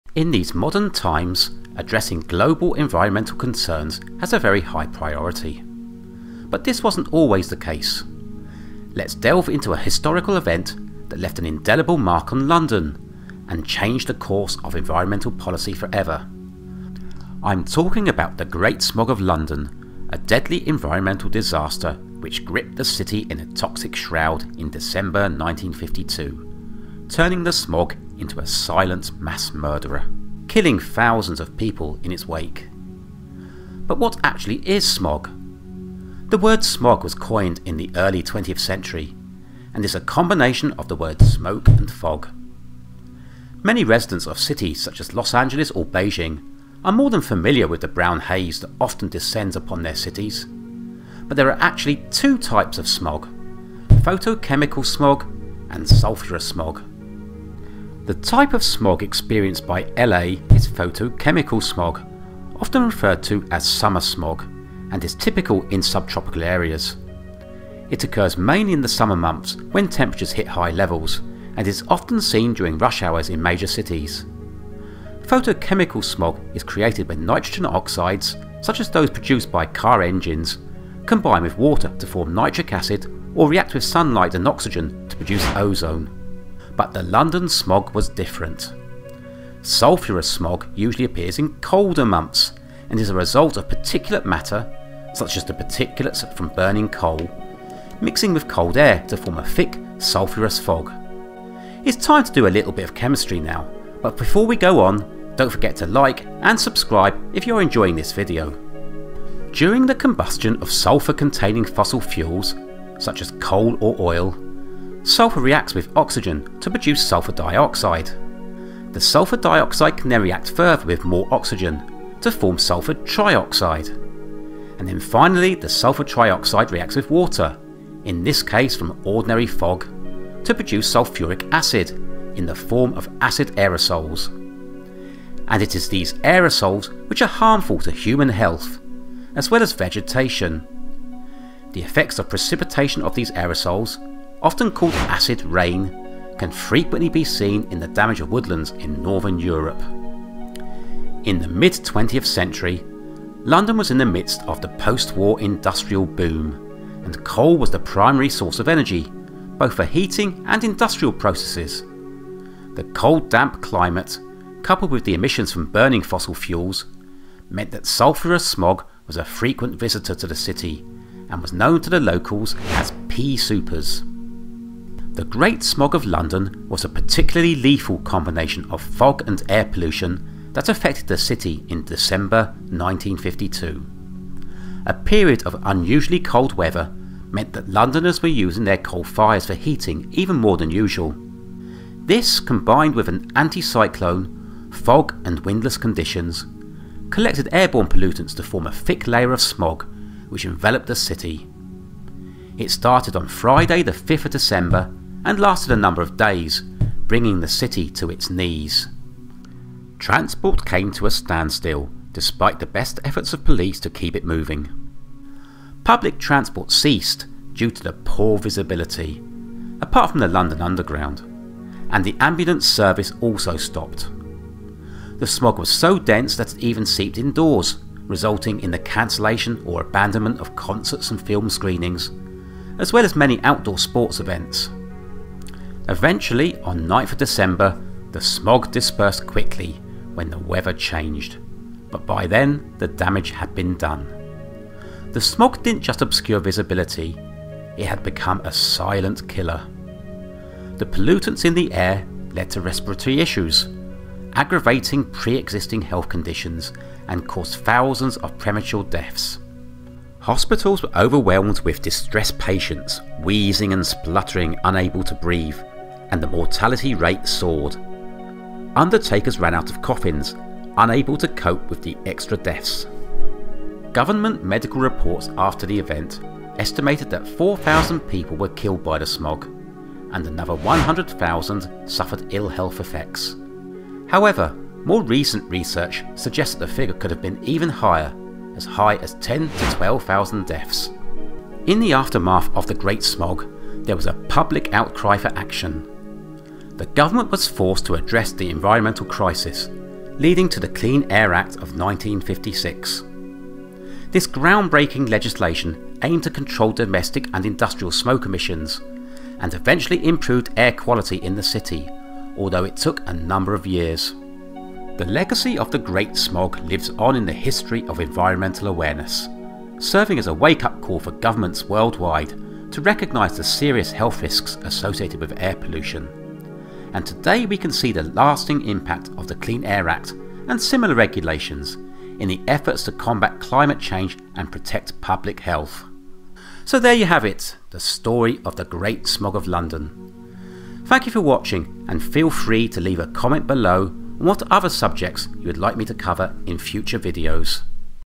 London’s Silent Killer : The Great Smog of 1952
Video about the Great Smog of London, a disastrous environmental event in 1952 that claimed the lives of 12000 people, what causes smog, and why it is deadly.